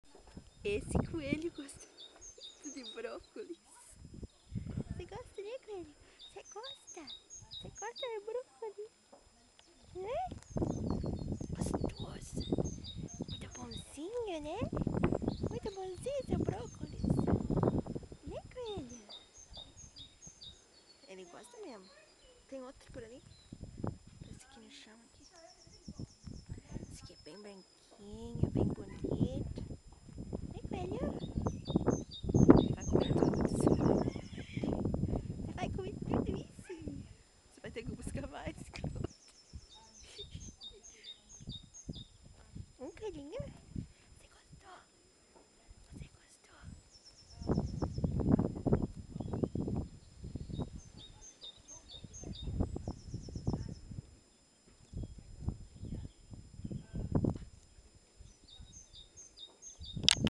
Beautiful Bunny Rabbit Munching Brocoli
Beautiful Bunny Rabbit Munching Brocoli In Hertfordshire, 3 April 2010.